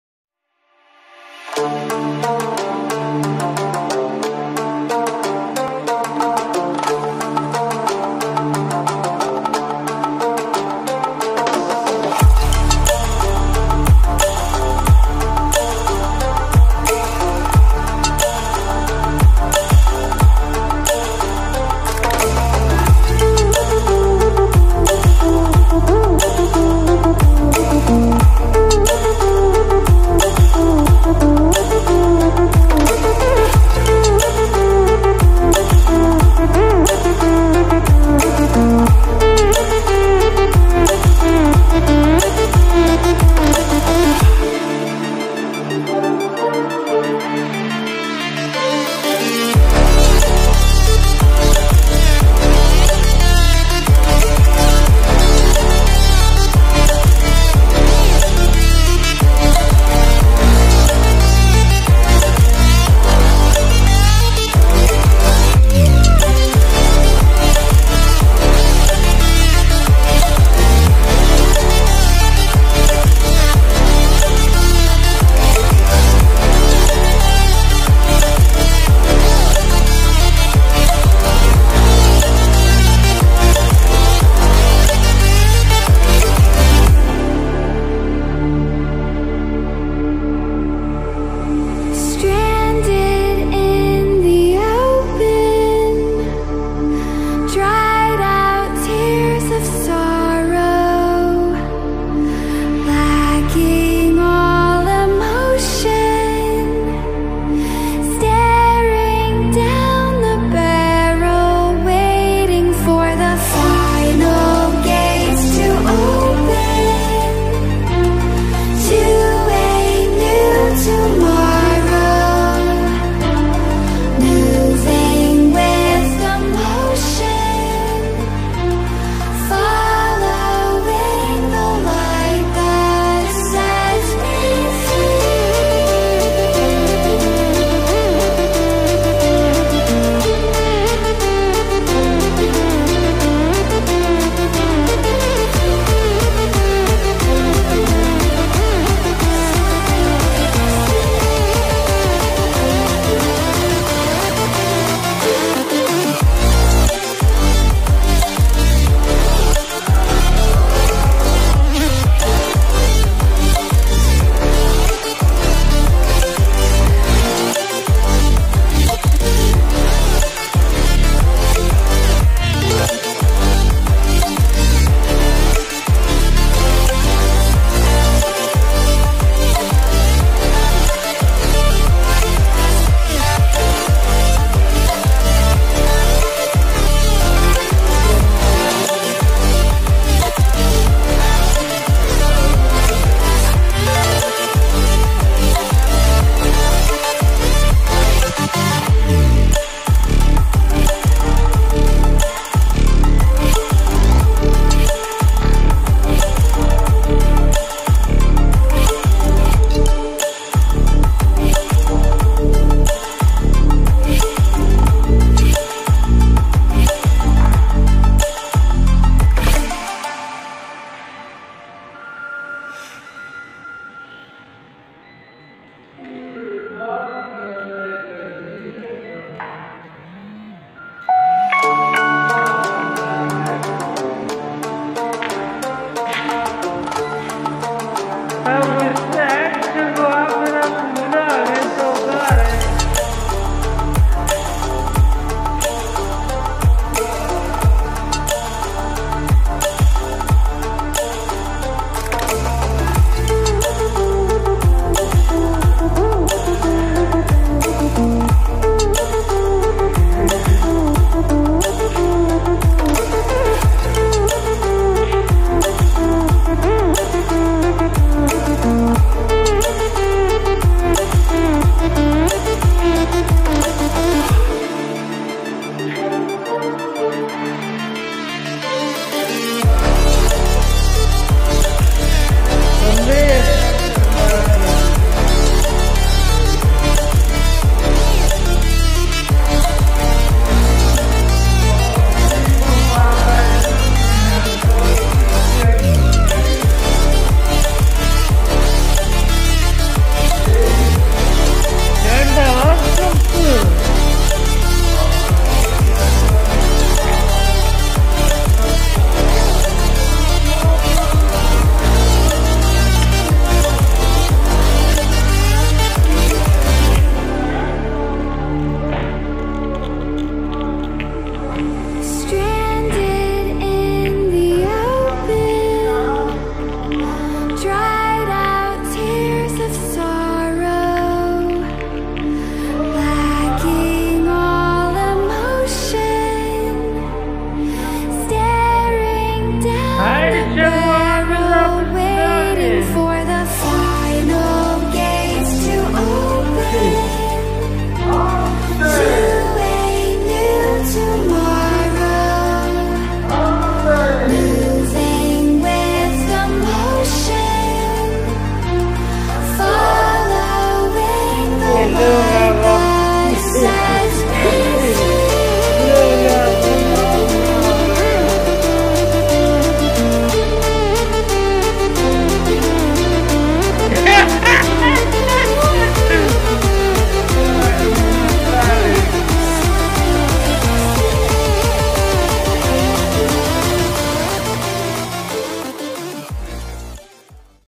Bike Riding In Rain |Bike Riding in Hills Pakistan|hard track in rain
in this video you can watch hard track in hill of kohe suleman.rain also is available and iam riding bike with my friend.hard track of north hills.